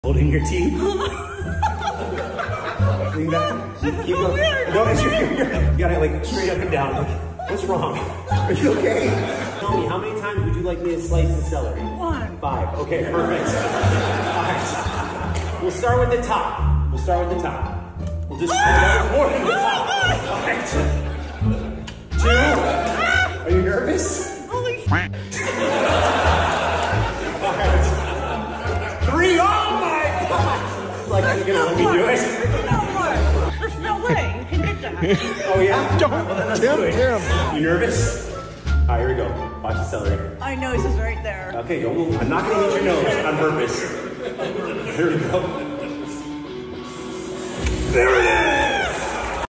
SCARY CELERY Trick Shot 😱
The Celery SLICER at Corporate Events!!! This was AWESOME! SHE was TERRIFIED!